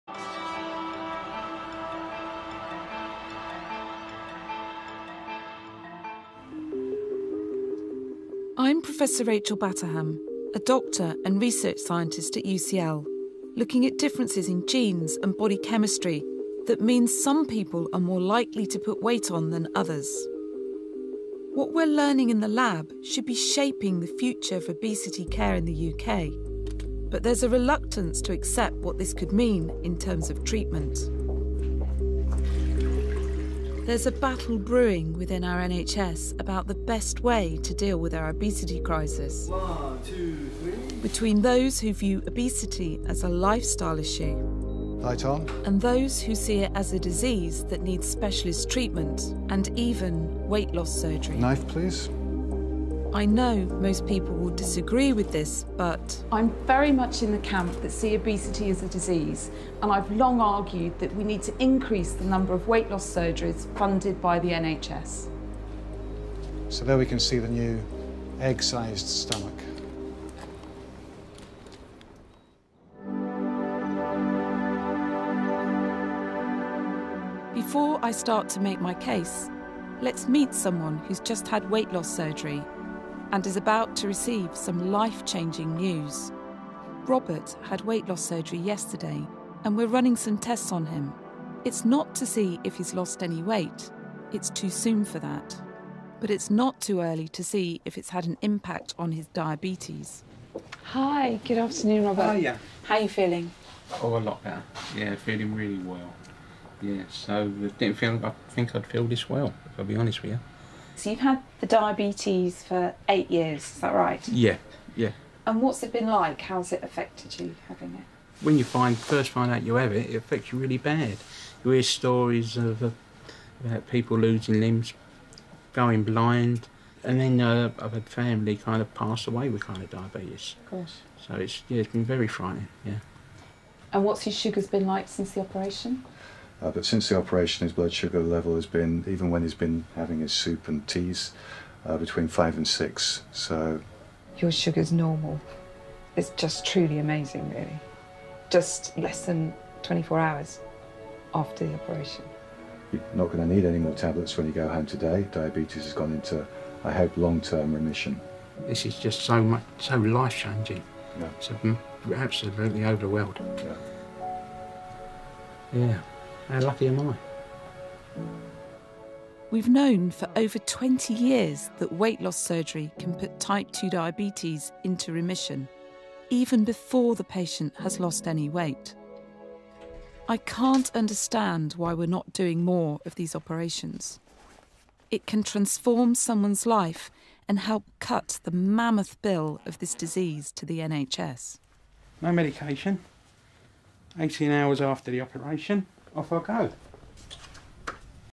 BBC documentary
Story of Bob, a taxi driver who I persuaded to consider bariatric surgery when we talked about his diabetes during the cab ride.